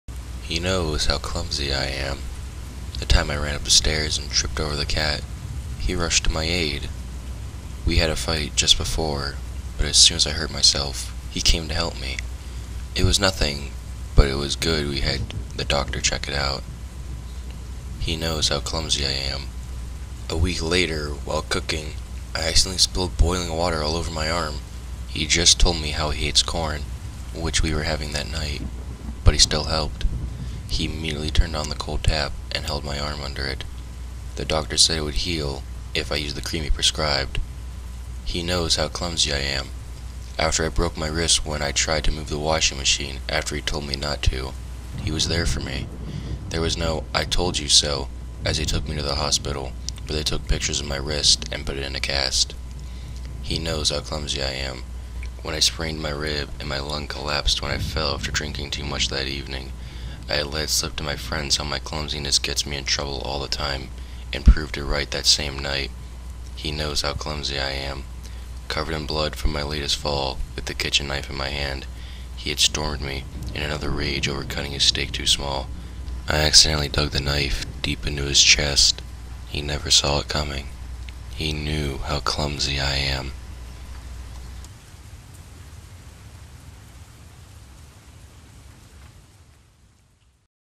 Creepy Pasta Reading -He knows how clumsy I am- made by Myrania
This story is like a little poem and I love it, and if you are being abused please talk to someone about it.

the background audio is rainfall